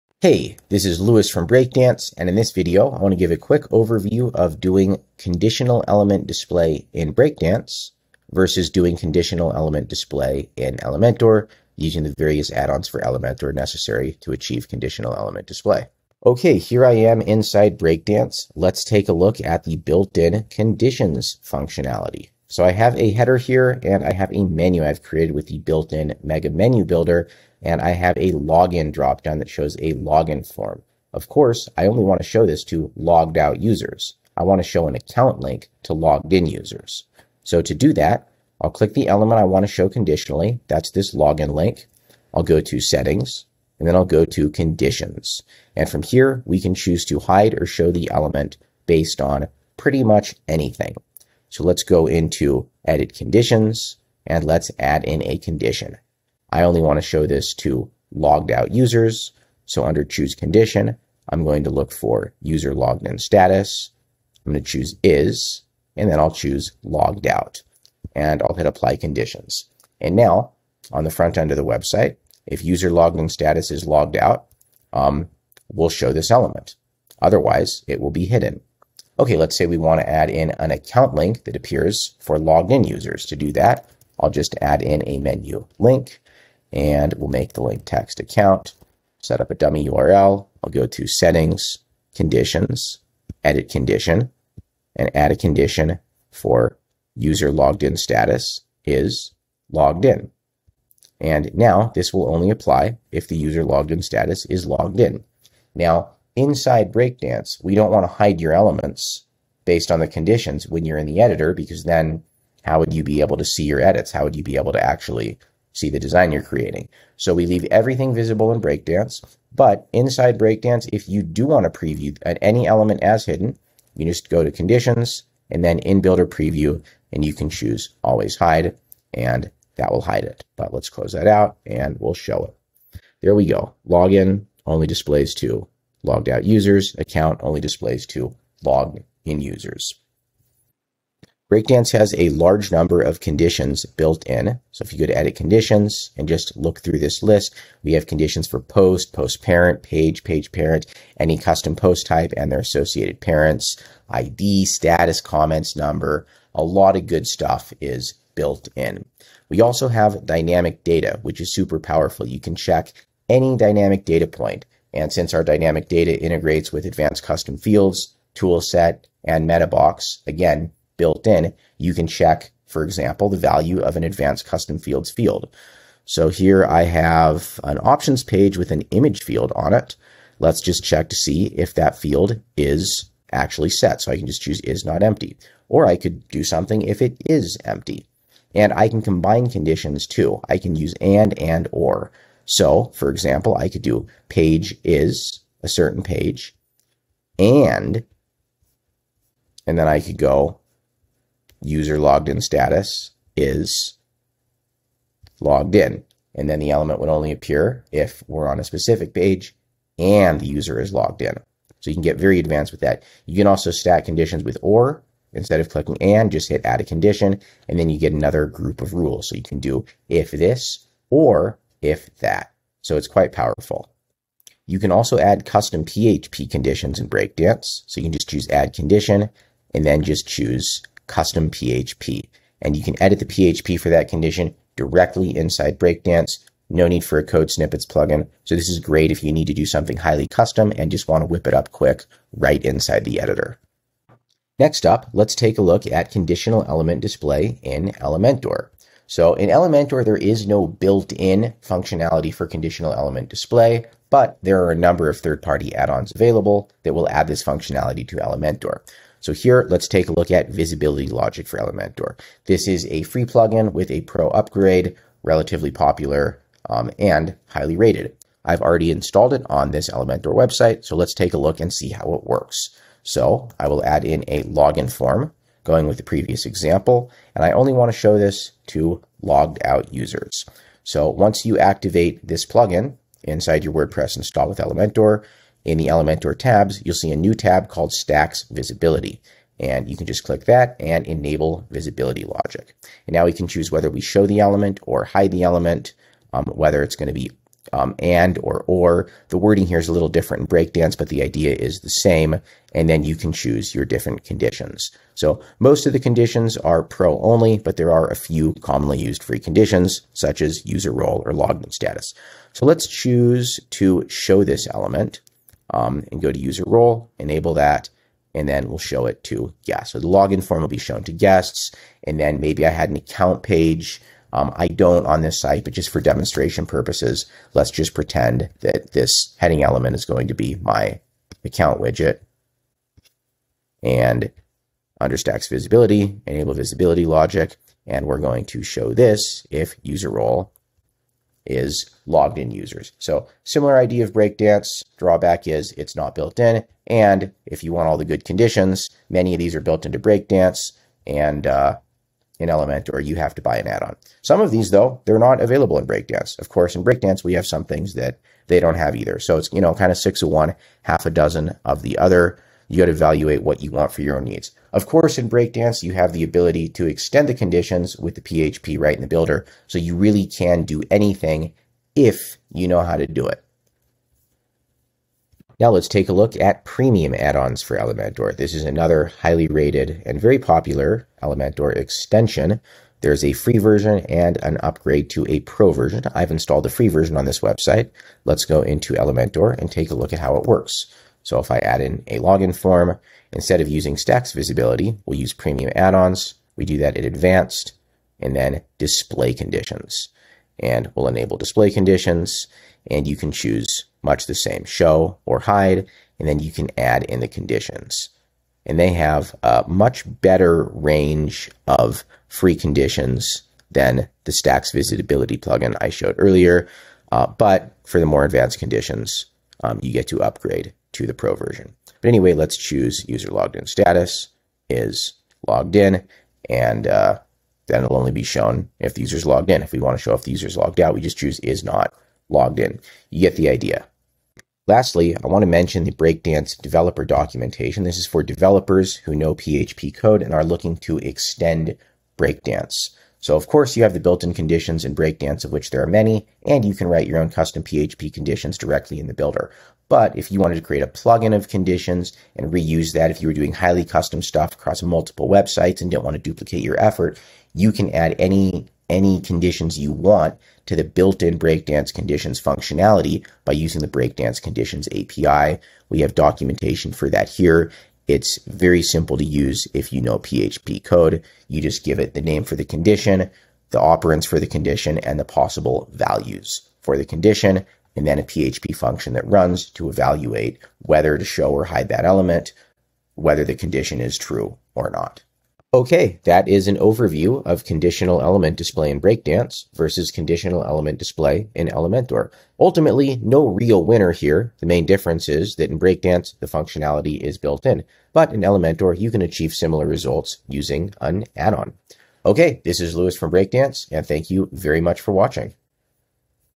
Conditional Element Display - Breakdance Vs Elementor
Learn and compare the conditional element display between Breakdance and Elementor.